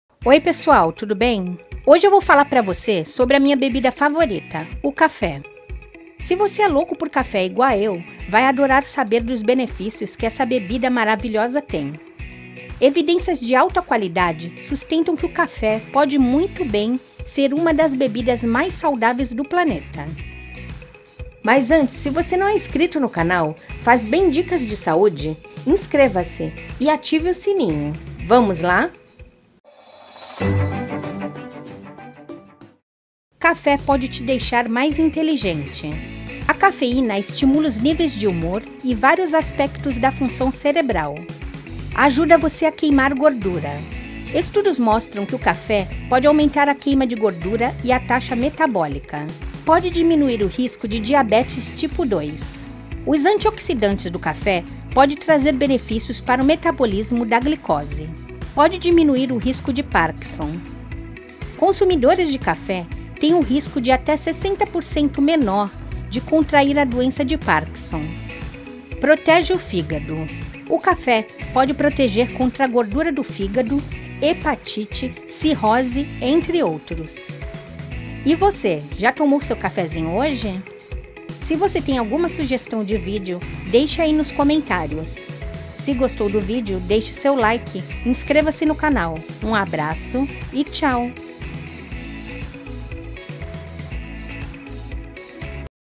Café é bom pra saúde? Ele Emagrece? Veja os Benefícios
Hoje vou falar pra você sobre minha bebida favorita, o café.
Se você é louco por café igual a eu, vai adorar saber dos beneficios que essa bebida  maravilhosa tem.
Evidências de alta qualidade sustentam que o café pode muito bem ser uma das bebidas mais saudáveis ​​do planeta.
Café pode te deixar mais inteligente A cafeína é estimulante por isso melhora os níveis de humor e vários aspectos da função cerebral.
Estudos mostram que o café pode aumentar a queima de gordura e a taxa metabólica

Gostou